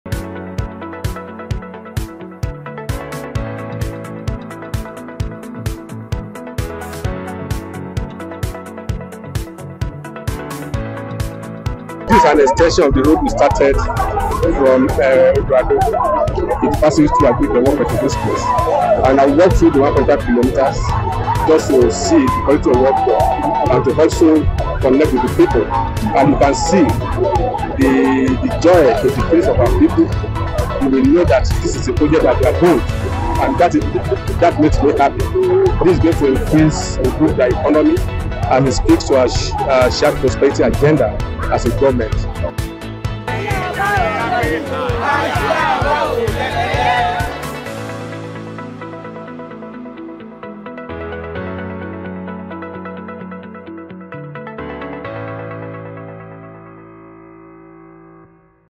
WATCH: Gov. Biodun Oyebanji inspected some ongoing projects in Ado Ekiti.
Gov. Biodun Oyebanji inspected some ongoing projects in Ado Ekiti, specifically the Agric Olope Market and Mofere - Ajilosun Road.